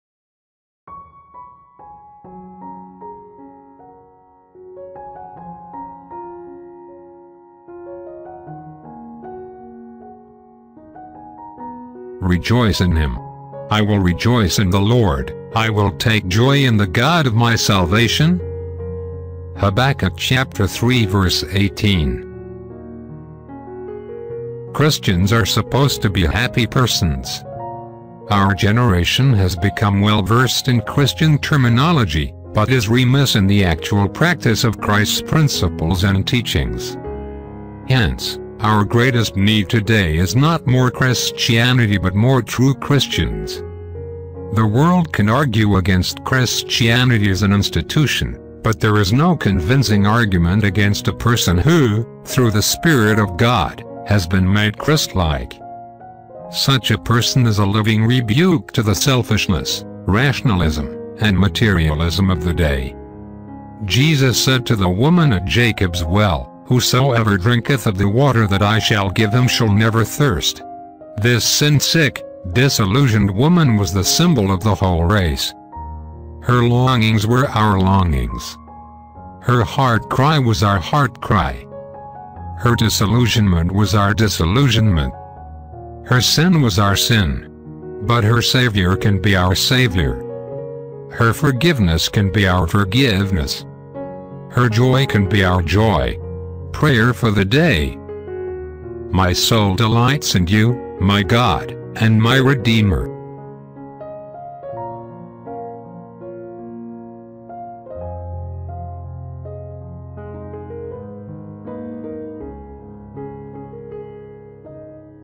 Rejoice in Him | DAILY BIBLE READINGS | January 16, 2021
Find the strength, peace and comfort you need to strengthen your relationship with God.

This is daily devotion is from Billy Graham. Please check their website, billygraham.org